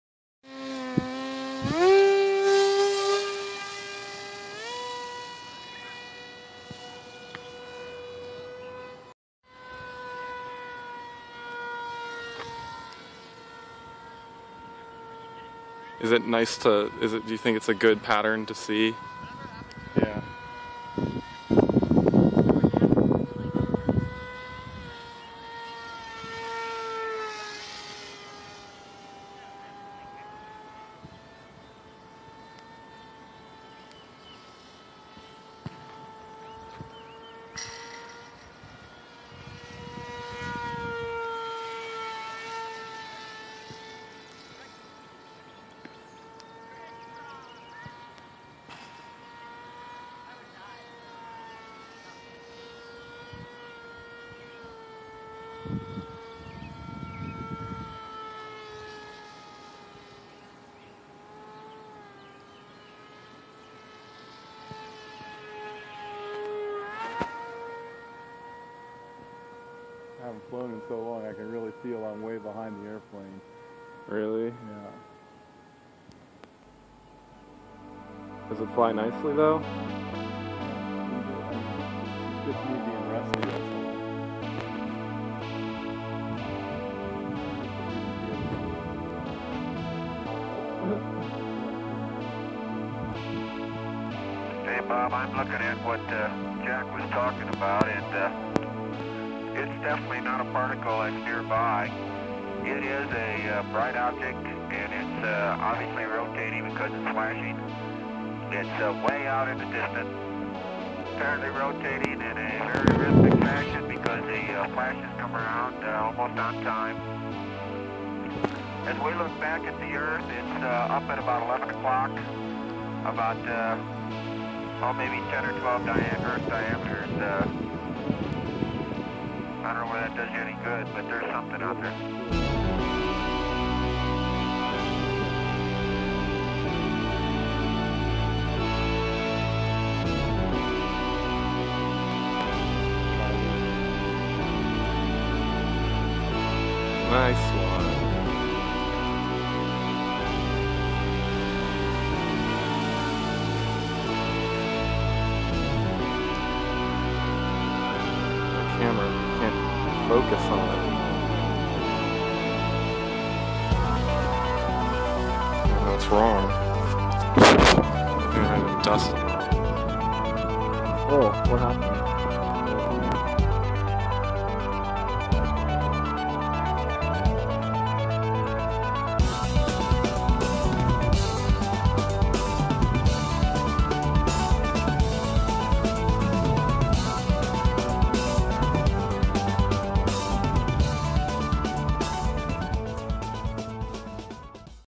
Zagi-400x Maiden Flight
Zagi that I built for a high school (JMHS) engineering project. Sorry for not being able to keep focus, it is too fast for my phone camera even at 3/4 throttle!